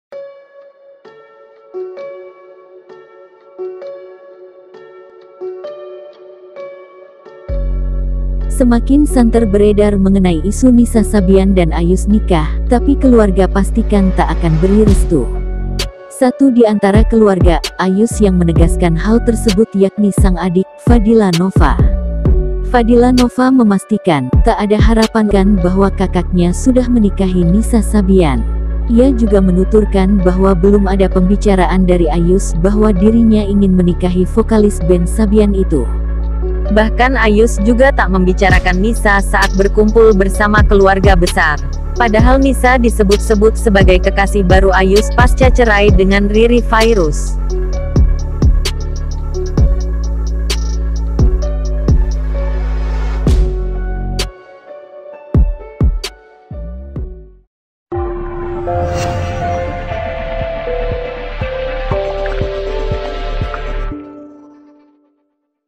ISU NISSA SABYAN DAN AYUS NIKAH, KELUARGA PASTIKAN TAK AKAN BERI RESTU
TRIBUNJAMBI.COM-Semakin santer beredar mengenai isu Nissa Sabyan dan Ayus nikah, tapi keluarga pastikan tak akan beri restu. Satu di antara keluarga Ayus yang menegaskan hal tersebut yakni sang adik, Fadhila Nova.

Fadhila Nova memastikan, tak ada harapan untuk kisah asmara Ayus dan Nissa Sabyan, untuk sampai ke pelaminan. Sebab, Fadhila menegaskan bahwa dirinya dan keluarga besar tak akan memberikan restu pada Ayus dan Nissa Sabyan jika keduanya jika hendak menikah.

Fadhila merasa sudah sangat tersentuh dengan sosok Ririe Fairuz yang pernah menjadi kakak iparnya.